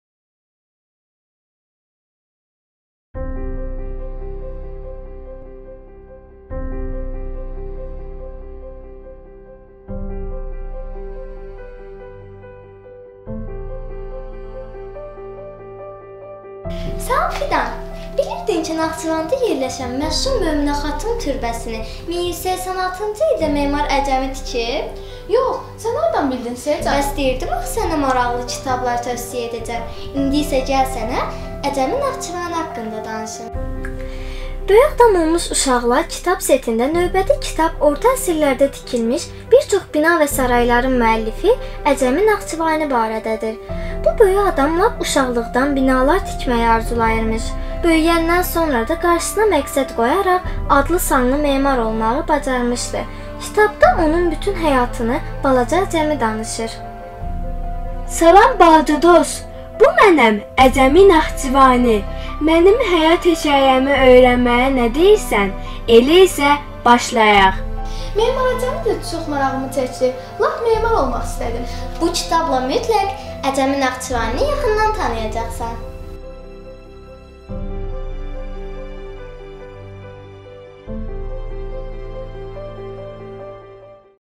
"Böyük adam olmuş uşaqlar" kitab seti - Memar Əcəmi
"Hədəf" nəşrlərinin təqdim etdiyi "Böyük adam olmuş uşaqlar" kitab seti tarixi şəxsiyyətlərin - xalqımızın qəhrəman və alim oğullarının həyatını uşaq personajların dilindən çatdırır.

Təqdimatçılar: Selcan və Fidan
Səsləndirmə: İnci